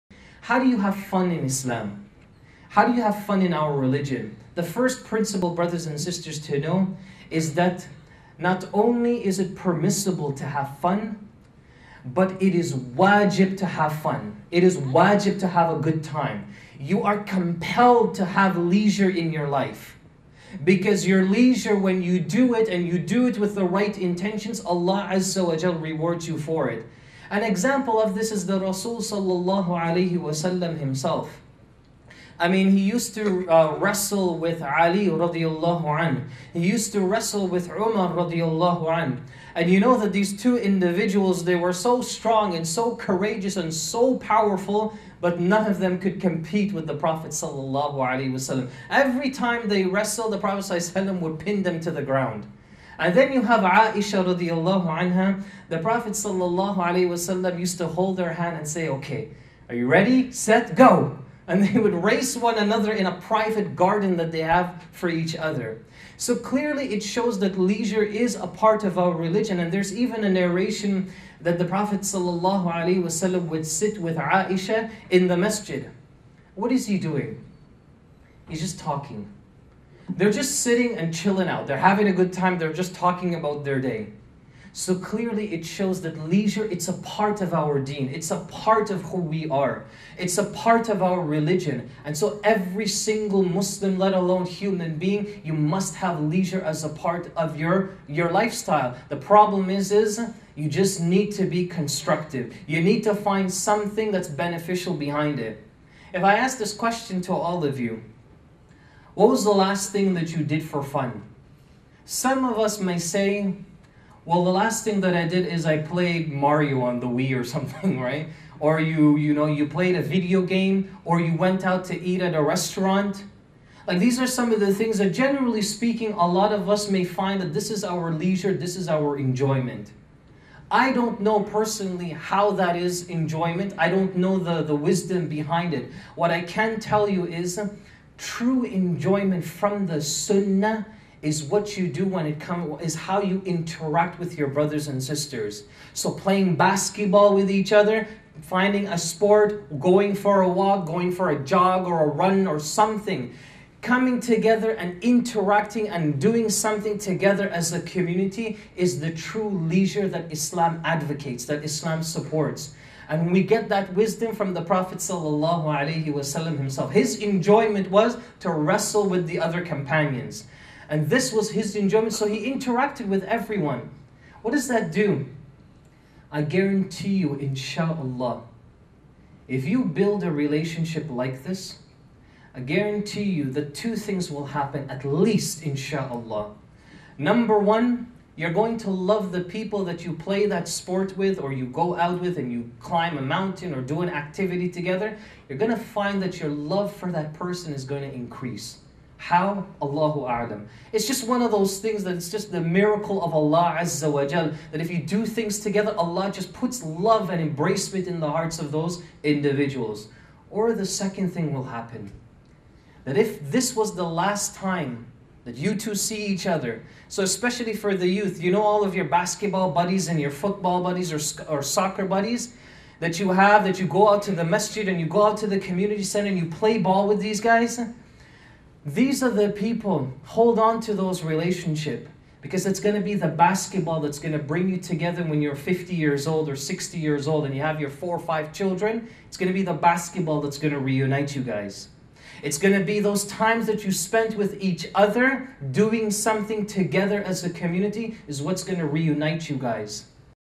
The Muslim Ideal Of Fun
How did the Prophet (SAWS) have fun? Sheikh Musleh Khan explains